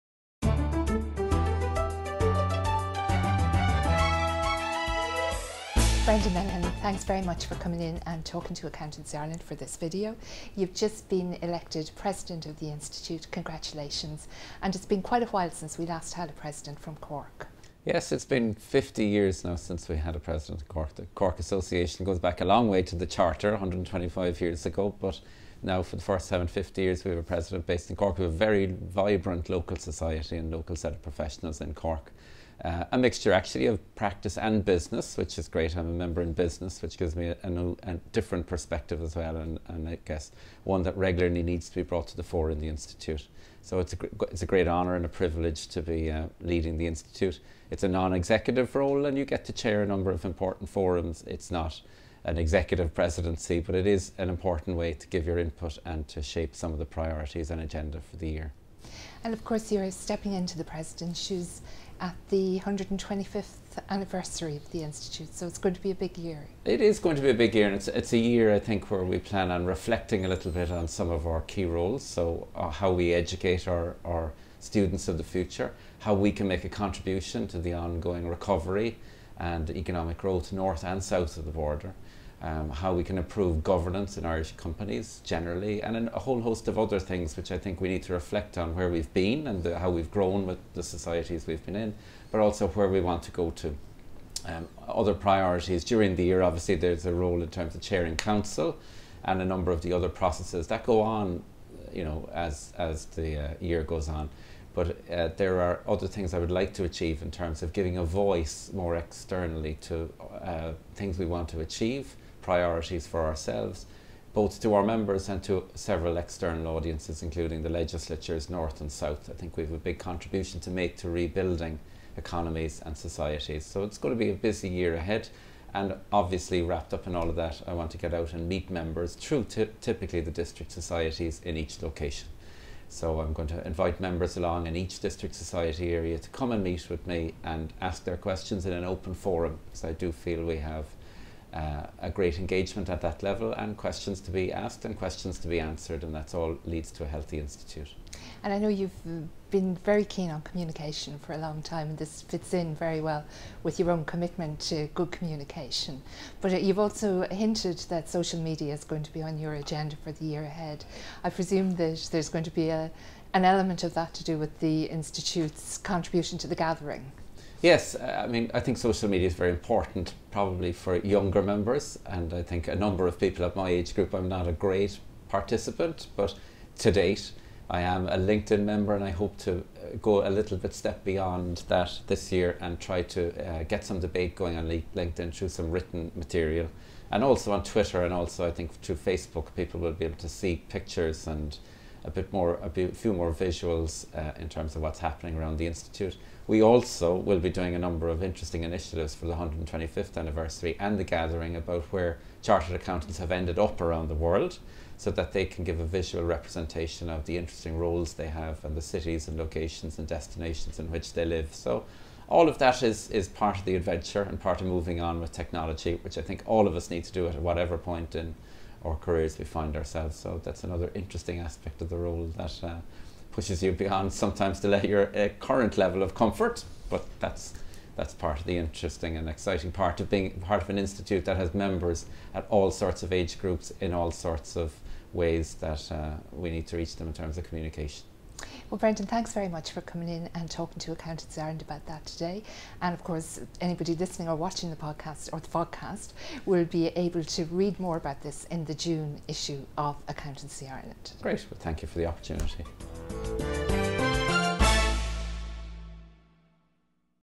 Accountancy Ireland June 2013 vodcast
Brendan Lenihan, FCA, was elected President of Chartered Accountants Ireland at the AGM on 17 May 2013. In an interview with Accountancy Ireland magazine he discussed his plans for his term of office, the role of the Institute in its 125th Anniversary year and its role in Irish Society. He also commented on communications with Members, the use of social media and contributions to the Gathering.